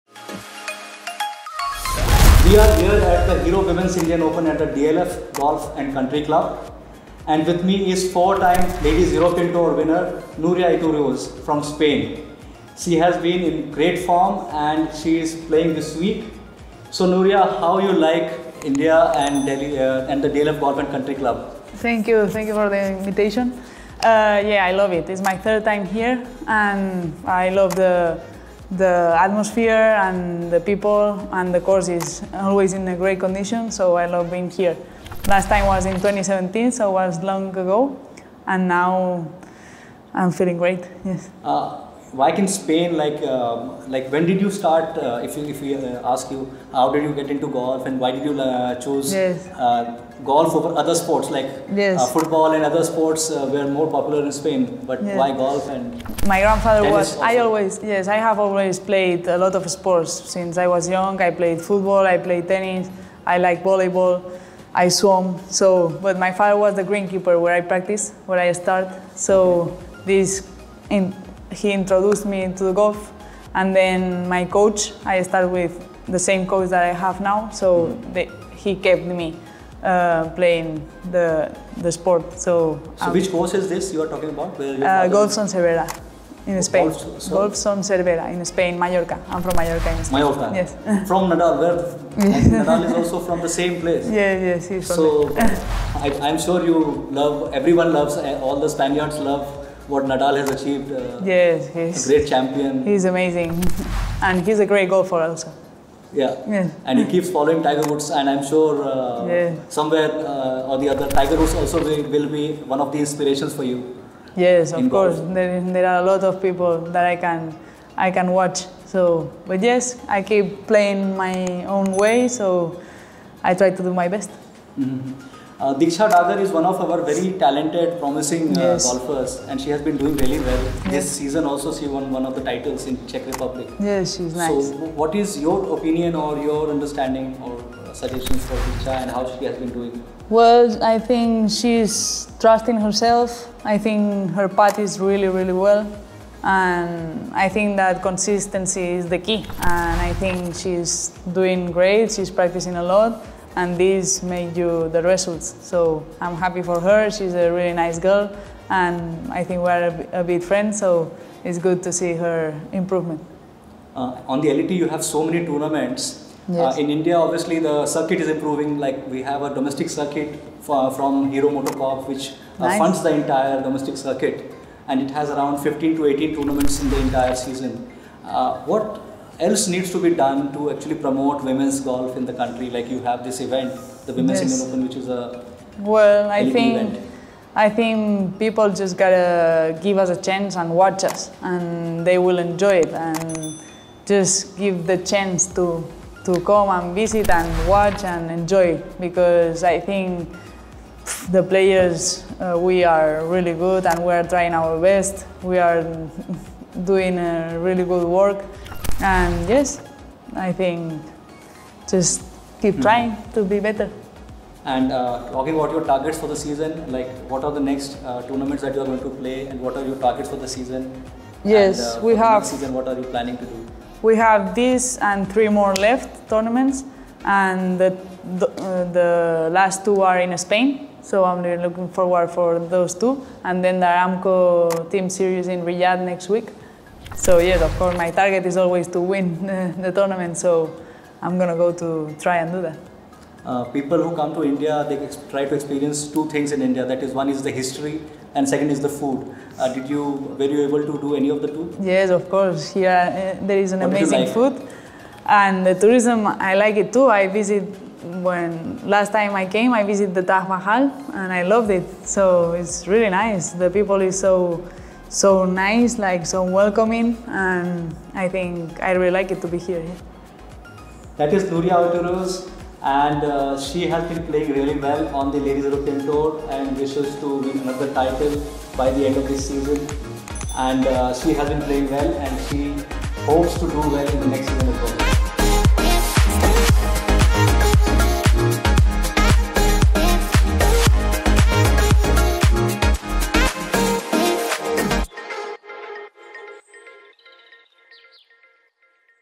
Nuria Iturrioz on why she chose golf over football and tennis
From the land of Rafael Nadal, golfer Nuria Iturrioz is a four-time winner in the Ladies European Tour. In a chat with Khel Now on the sidelines of the Women's Indian Open, Nuria revealed why she chose golf over other sports, her thoughts on India's Diksha Dagar and the things she loves about India.